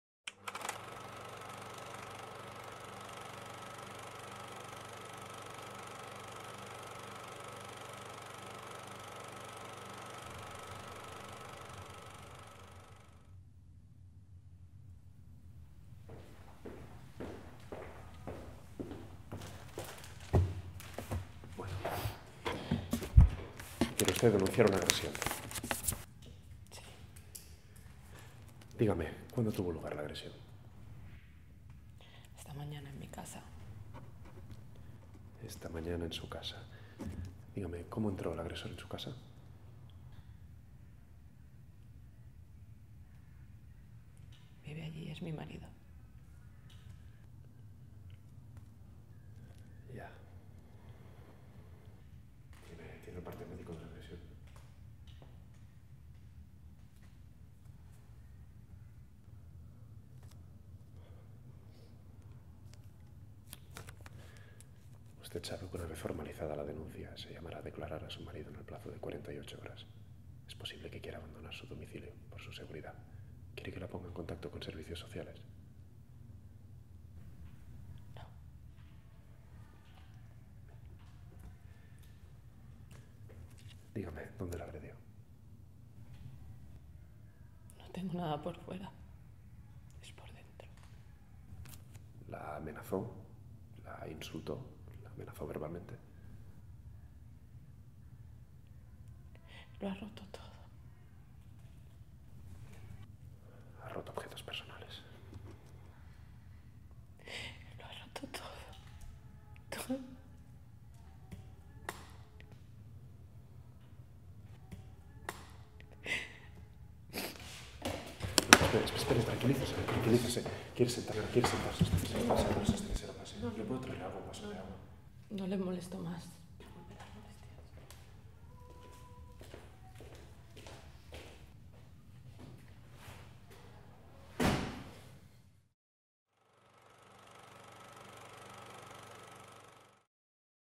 'Te doy mis ojos' Escena dirigida per Judith Colell. Postgrau El Timbal
El Timbal, centre de formació i creació escènica. Postgrau d'interpretació en cinema i televisió.
Escenes dirigides per Judith Colell. Els alumnes Víctor Cintero i Aleyda Puerto, en el seu treball a partir d'una escena de la pel·lícula 'Te doy mis ojos' de Icíar Bollaín.